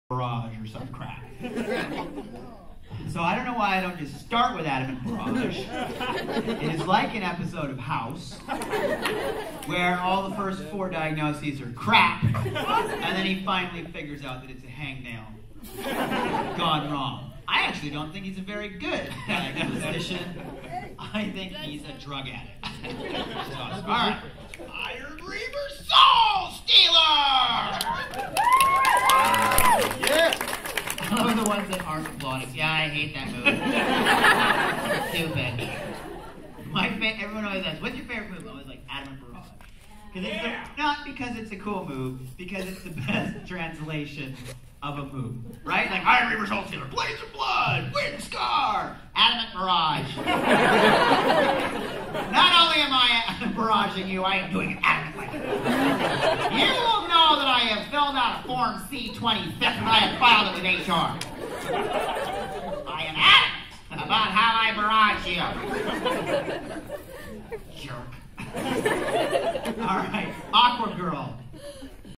March 10, 2013 Toronto Comic Con Pt. 5, "Iron Reaver Soul Stealer!"
Richard complaining about Inuyasha's most ineffective move, and how it's like an episode of House.